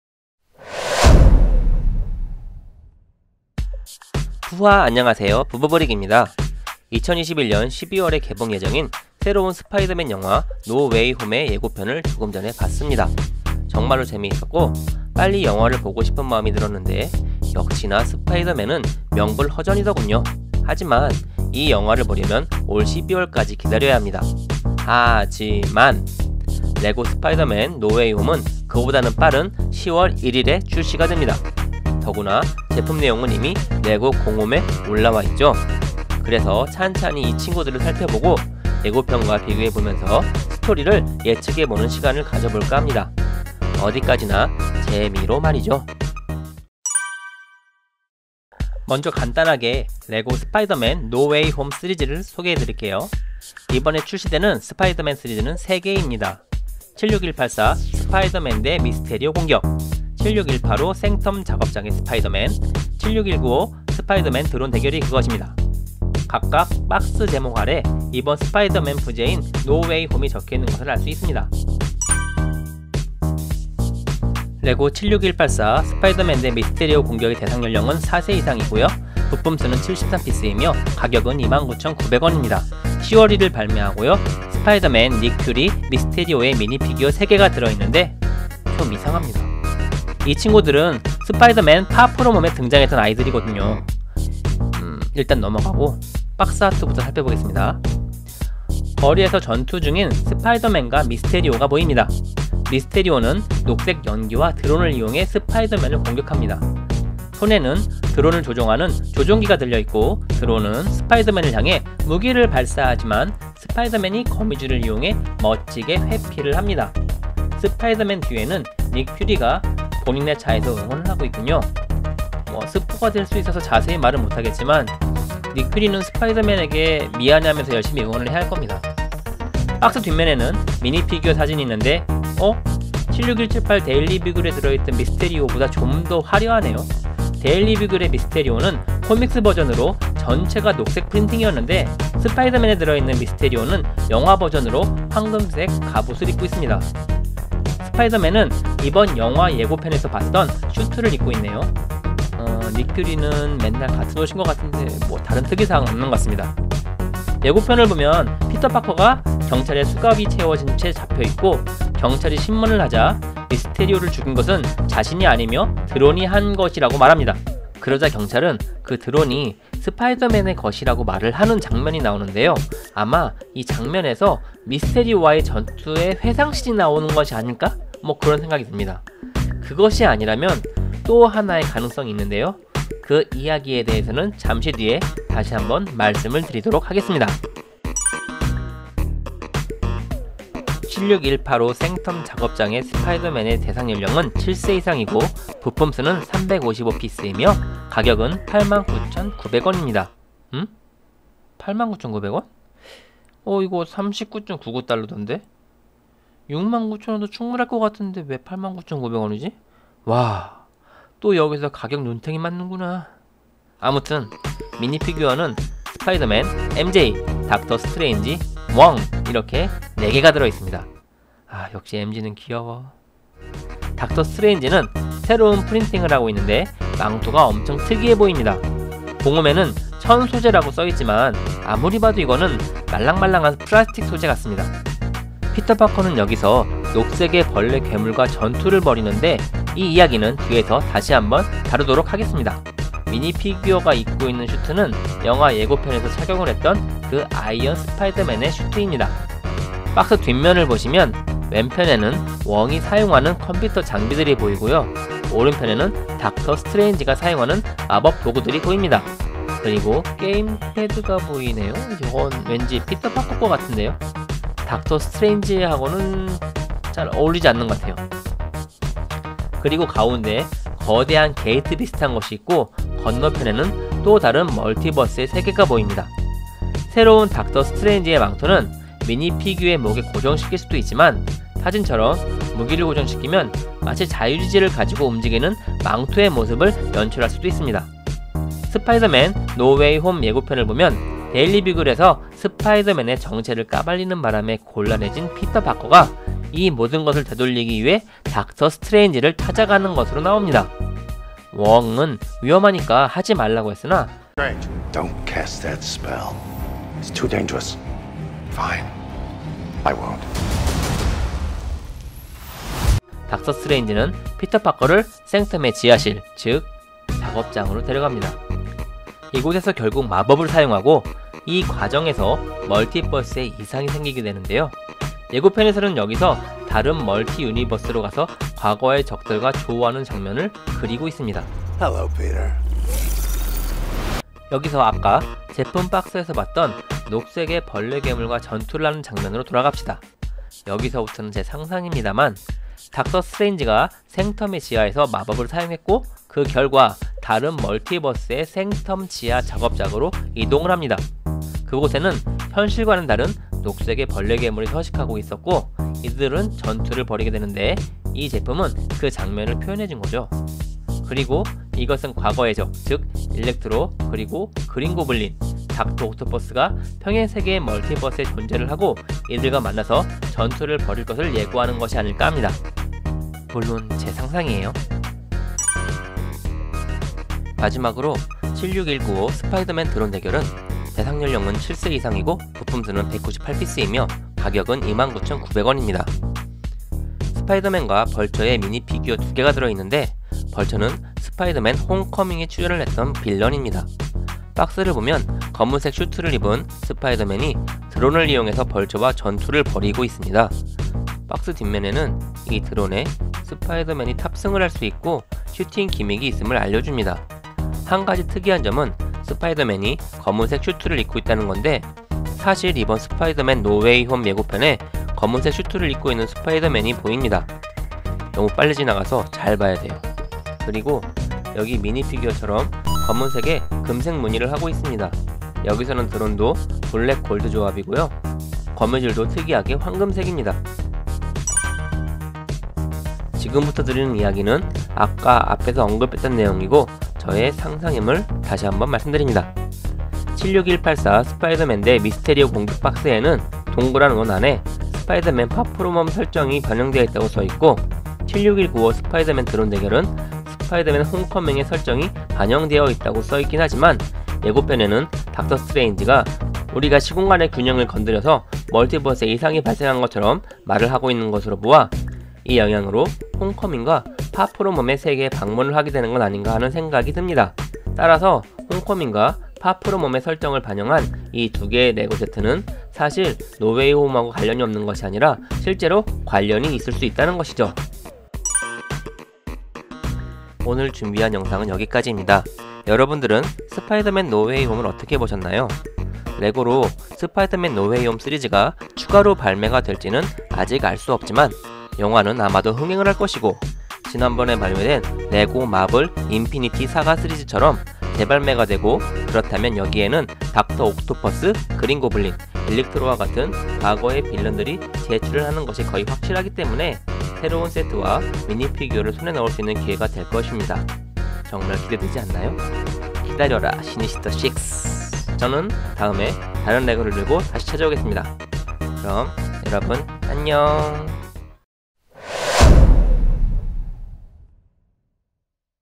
"스파이더맨 : 노 웨이 홈" 레고로 미리 보기 : LEGO Marvel Spider-man No way home
12월에 개봉하는 스파이더맨 : 노 웨이 홈 미리보기 입니다.

10월1일에 출시되는 3개의 레고 스파이더맨 : 노 웨이 홈 시리즈를 통해 스토리를 예측해 보았습니다.

레고 마블 76184 스파이더맨 대 미스테리오 공격
레고 마블 76185 생텀 작업장의 스파이더맨
레고 마블 76195 스파이더맨 드론 대결